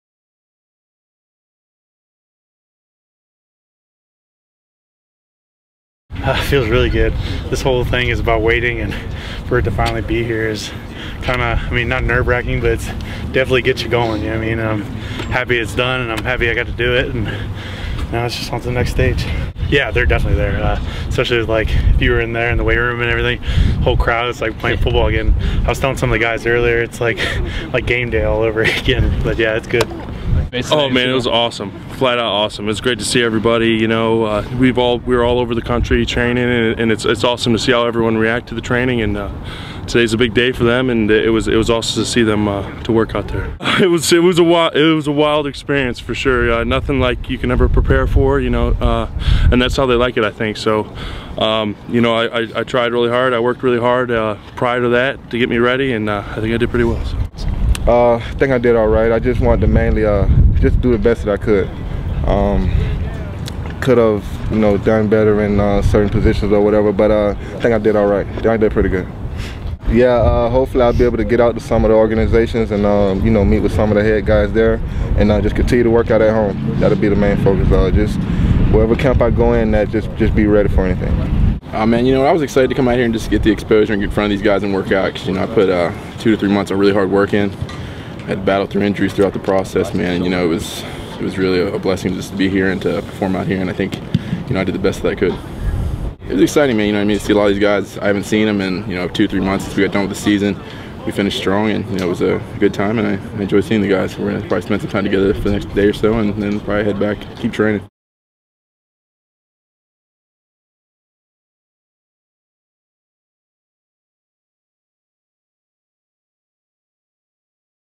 2013 Ohio Football Pro Day
Former Ohio Football student-athletes Skyler Allen, Eric Herman, Tremayne Scott, and Neal Huynh speak about their experiences at Ohio Football's 2013 Pro Day at Peden Stadium.